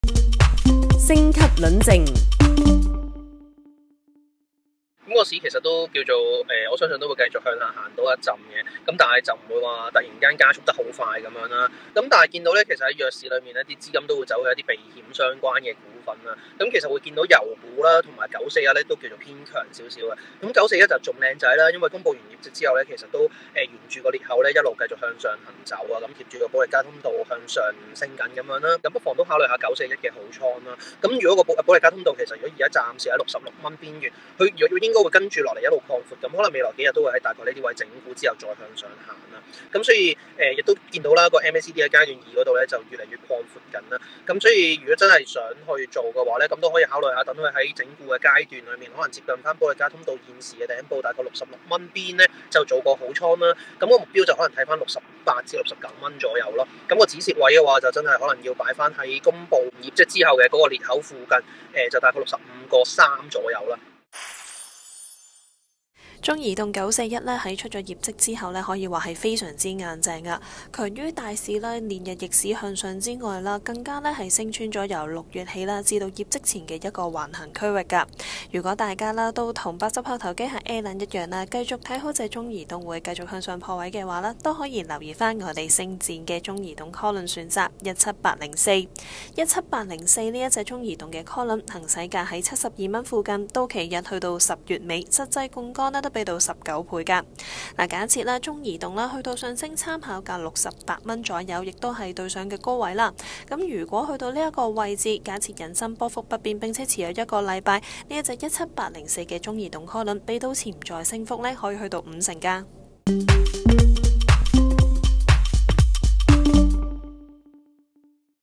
【星級輪證】弱市避險搏強勢股 中移動好倉部署
星級嘉賓 八十後投機客 指大市會繼續向下，資金避險走向強勢股，中移動屬首選，料保歷加通道會繼續擴濶，有機衝上上次高位。好倉部署可以考慮星展中移CALL輪17804，潛在升幅達五成。
相關輪證可以留意：(17804）中移星展三十購
本周星級嘉賓：
14/8 紅磡索螺絲
15/8 豹姐
16/8 八十後投機客
17/8 唐牛

節目內容只屬主持及嘉賓意見
並非本公司立場或買賣建議
投資涉及風險，資產價格可升可跌